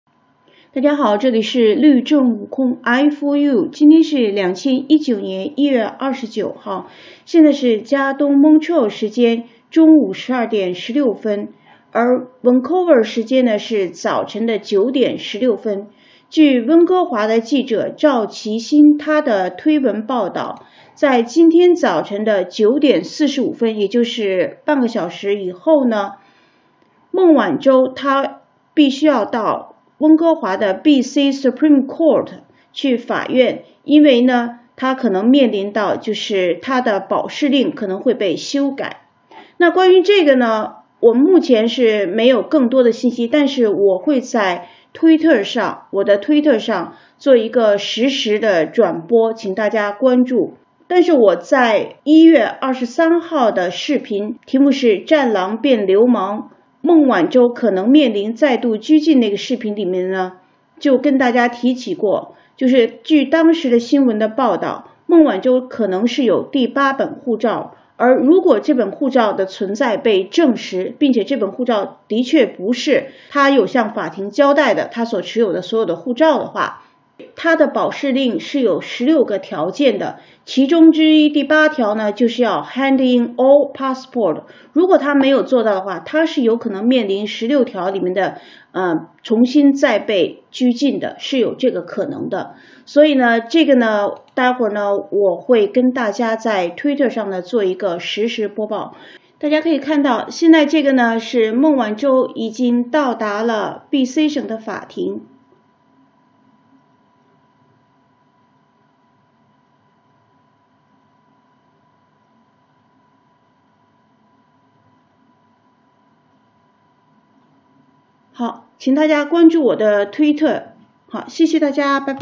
20190129-1 【🔥🔥🔥孟晚舟再度出庭！】🔥🔥🔥实时播报在律政悟空推特！
更新🔥🔥🔥请注意：📢📢📢今天的庭讯是孟晚舟方面提出的请求，更换一个担保人. ”